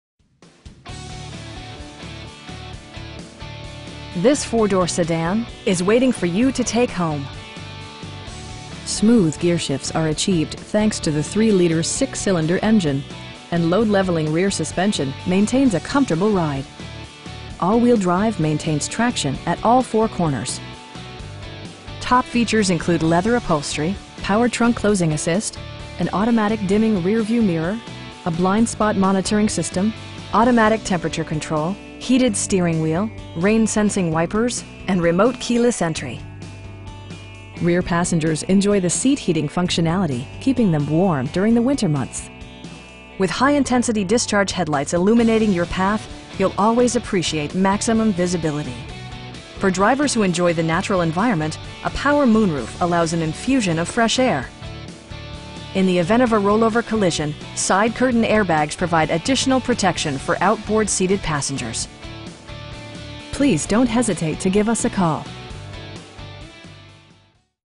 2015 Jaguar XJ XJL Portfolio AWD in Madison, NJ 07940
Madison Jaguar

This 4 door sedan is waiting for you to take home! Smooth gearshifts are achieved thanks to the 3 liter 6 cylinder engine, and load leveling rear suspension maintains a comfortable ride. All wheel drive maintains traction at all four corners. Top features include leather upholstery, power trunk closing assist, an automatic dimming rear-view mirror, a blind spot monitoring system, automatic temperature control, heated steering wheel, rain sensing wipers, and remote keyless entry. Rear passengers enjoy the seat heating functionality, keeping them warm during the winter months. With high intensity discharge headlights illuminating your path, you'll always appreciate maximum visibility. For drivers who enjoy the natural environment, a power moon roof allows an infusion of fresh air. In the event of a rollover collision, side curtain airbags provide additional protection for outboard seated passengers. We know that you have high expectations, and we enjoy the challenge of meeting and exceeding them! Please don't hesitate to give us a call.